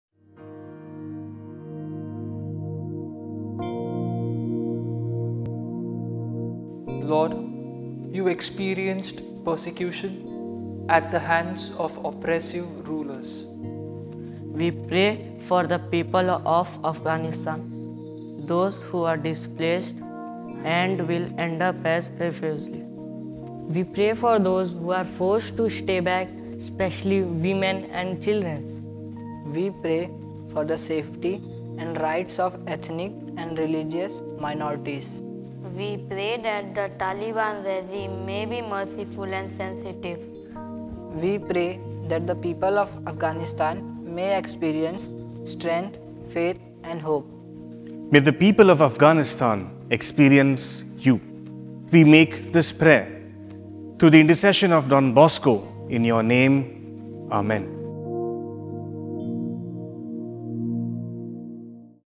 A Prayer for Afghanistan
Join us as we pray for the people of Afghanistan who find themselves in the midst of turmoil and uncertainty.